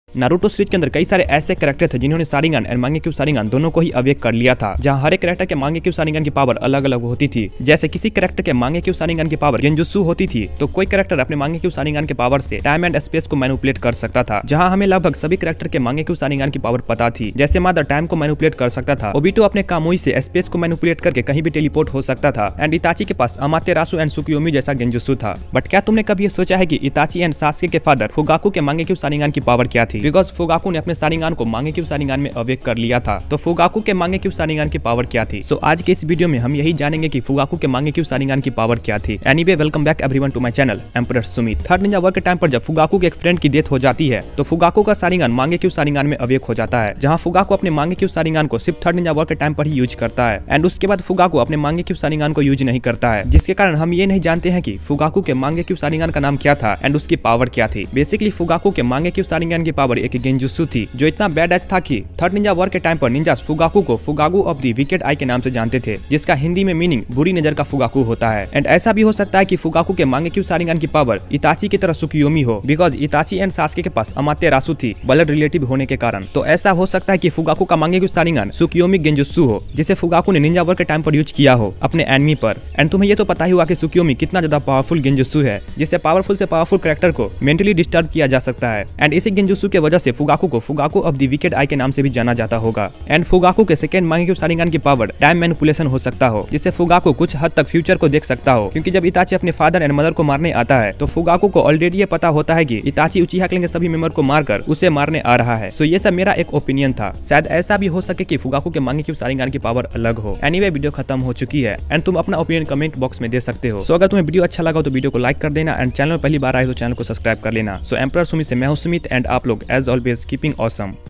Fugaku Ke Mangekyou Saringan Ki Power Kya Thi || Fugaku Ka Mangekyou Saringan Kya Tha
This video making only one purpose to grow anime community.💌 So please support all anime community YouTuber.😊

Fugaku Ka Mangekyou Saringan Kya Tha in hindi